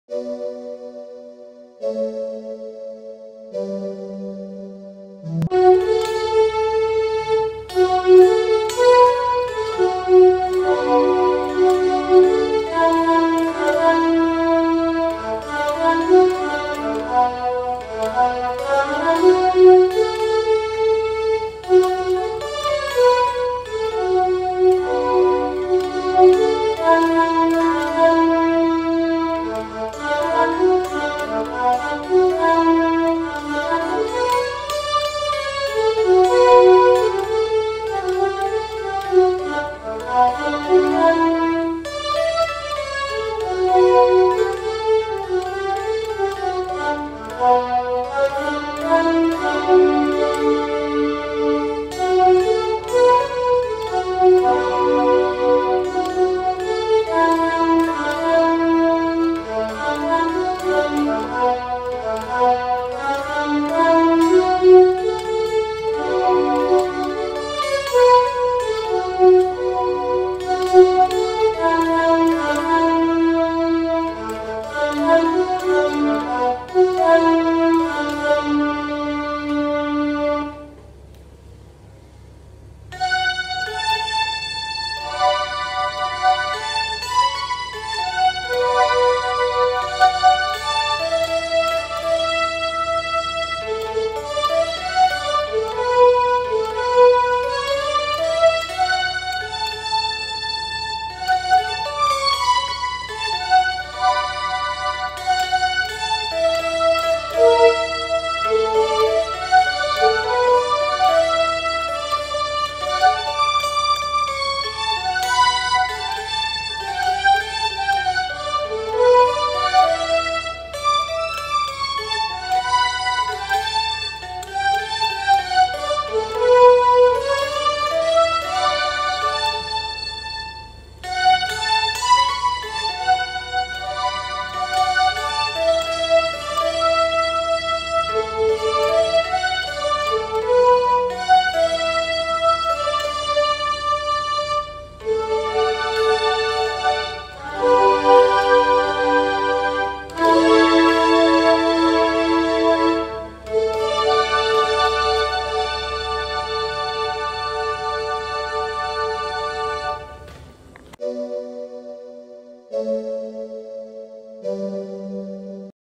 2 OCTAVE STRINGS PLAYED IN GUITAR
Hi. This is a demonstration melody to show how to play 2 octave strings by using guitar and guitar processor. The guitar playing techniques are basically same while playing but to get strings effect, lead and rhythm has to be played together. So it is a mix playing between Lead guitar and Rhythm guitar. Sometimes playing has to be paused till the end chords get full gain like actual keyboard. Keep Listening.!

Scale-Dmajor

thanks